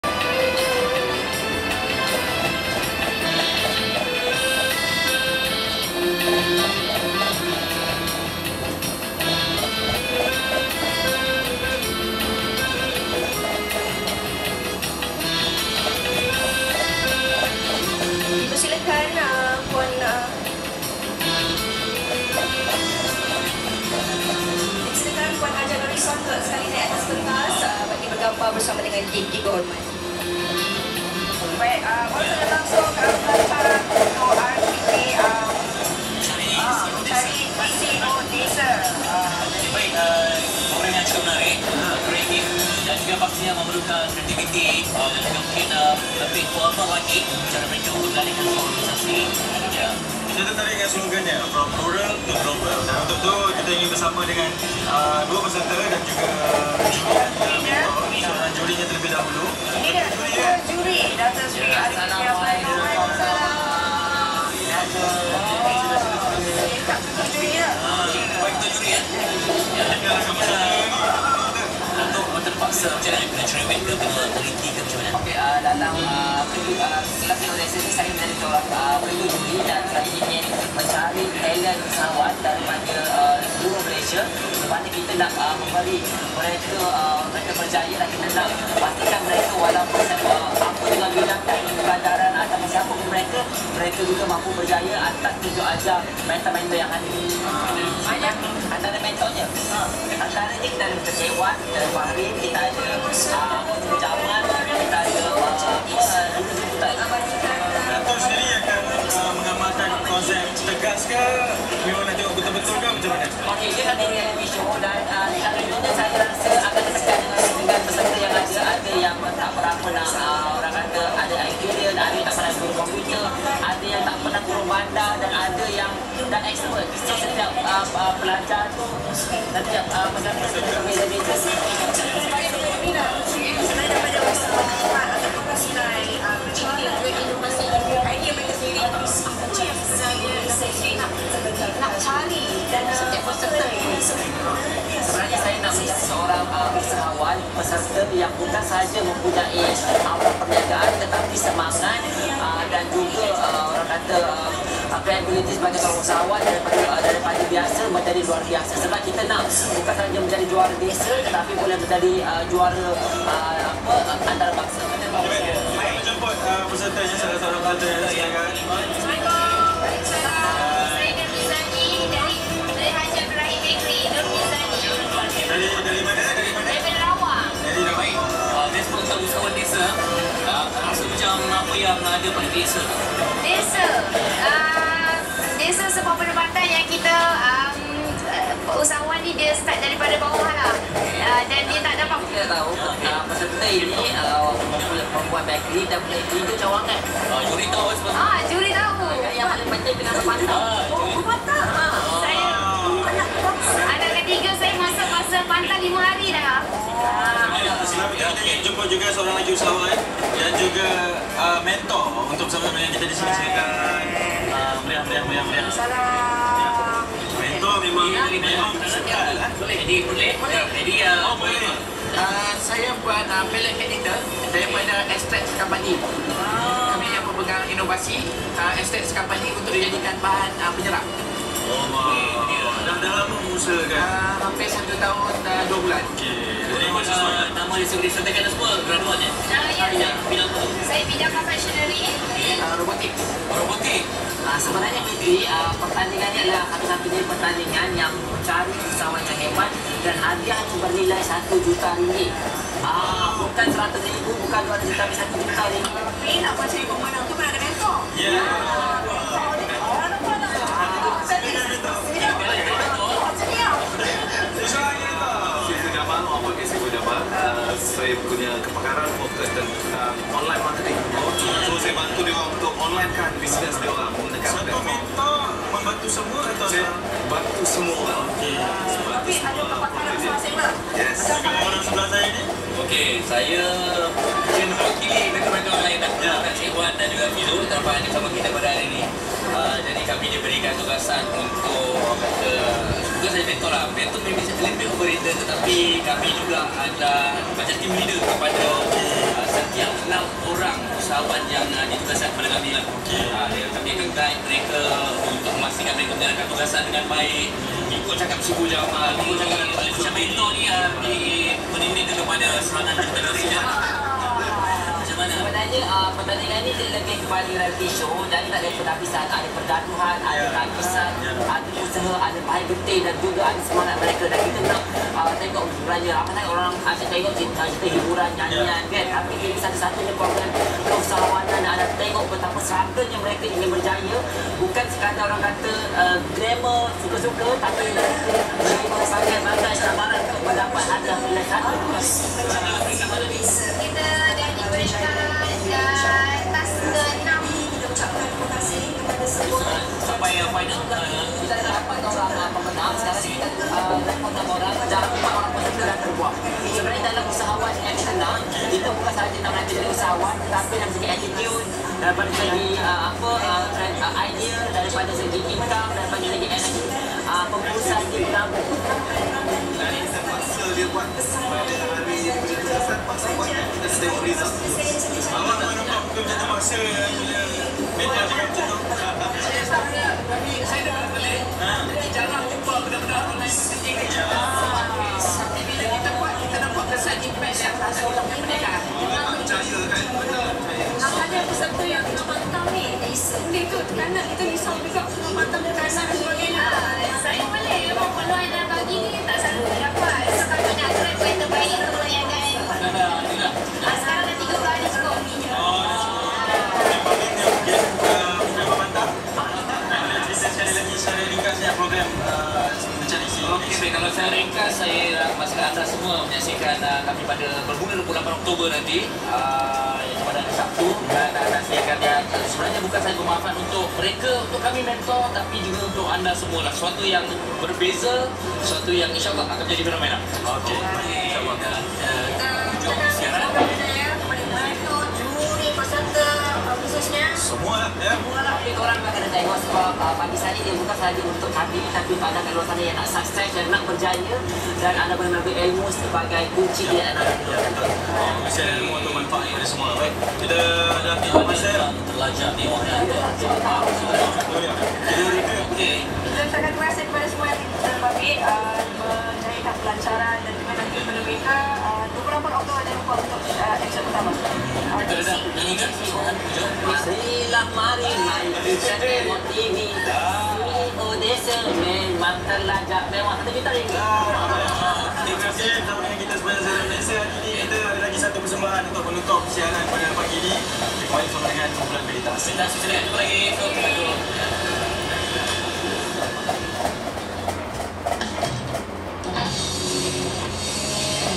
Pelancaran Program Realiti CEO Desa 2017
Majlis Pelancaran Program Realiti CEO Desa di Seri Pentas TV3.

Program ini akan disiarkan bermula pada 28 Oktober 2017 di TV3, setiap hari Sabtu, jam 5:30 petang.